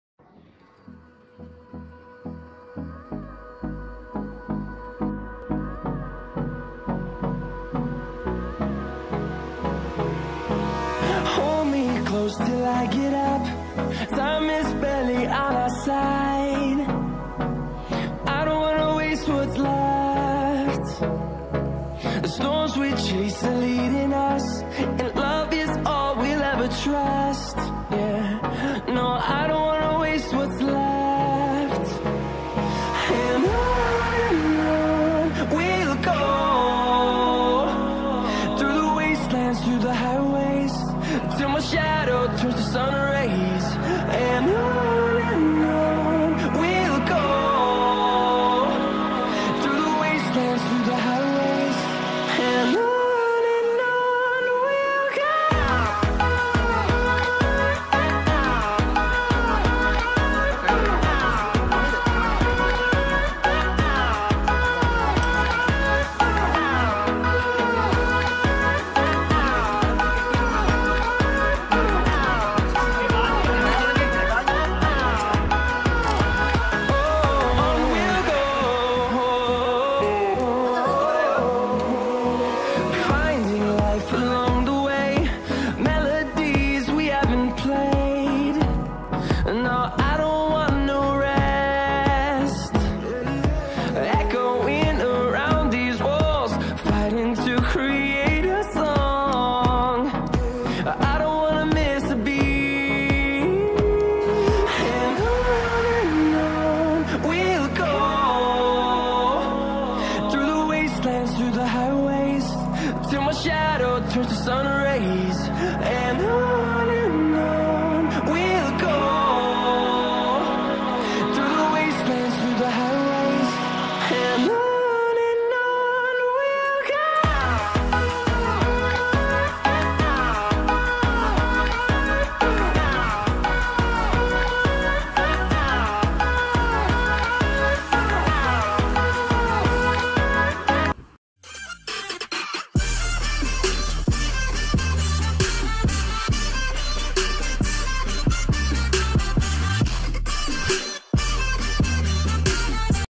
Line follower Robot
arduino line follower robot,

Line follower Robot-Line follower is an autonomous robot which follows either black line in white are or white line in black area.